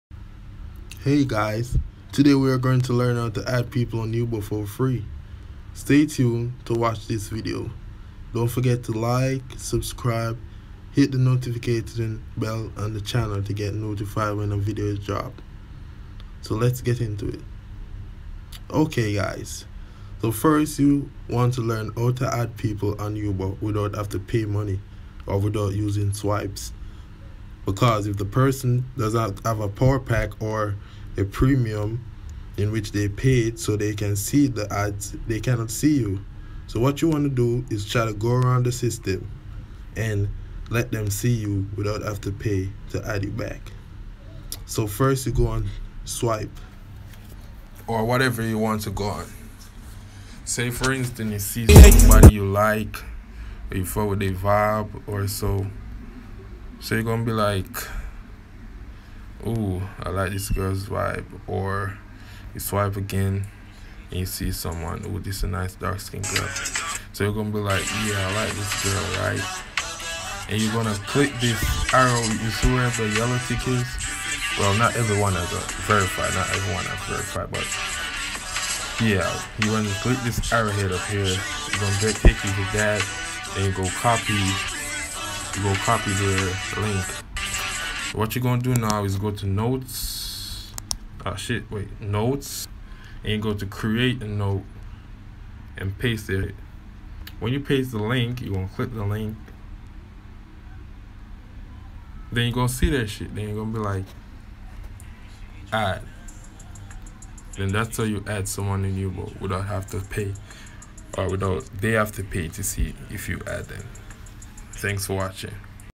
TRICK FOR YUBO!!!! How To Add Swipes For Them To See Without Paying Money!!! Totally Free!!!
This is very legit!

gang🛸💕🌙🎶